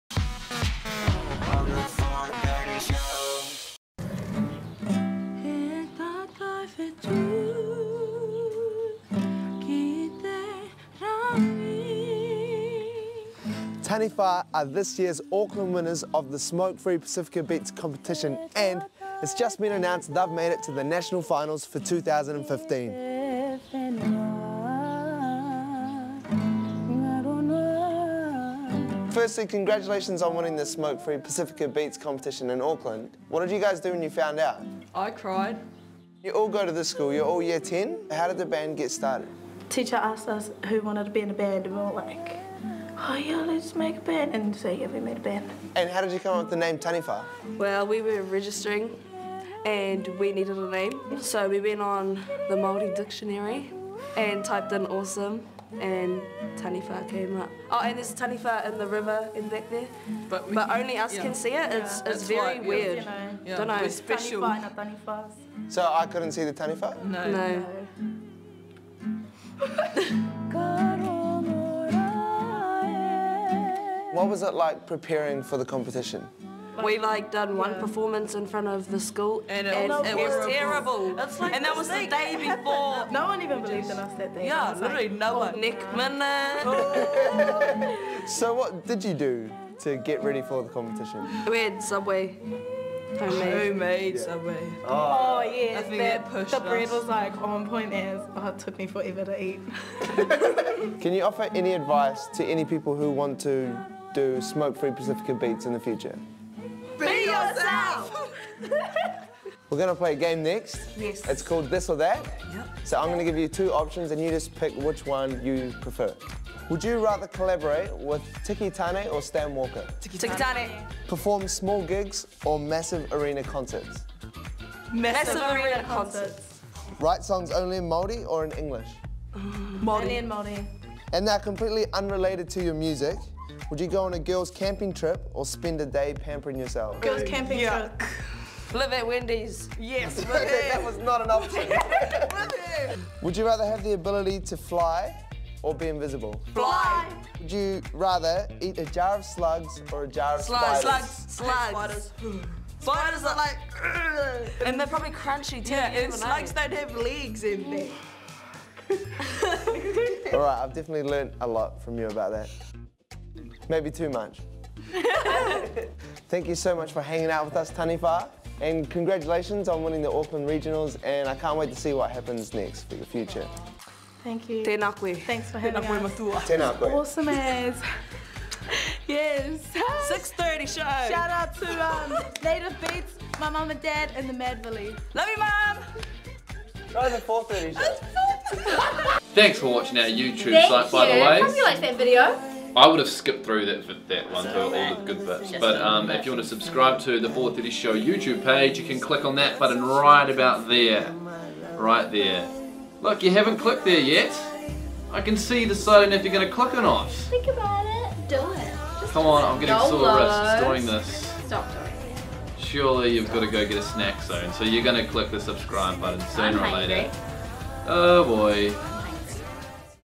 Brock checks out Taniwha
Brock checks out the soulful beat of Taniwha.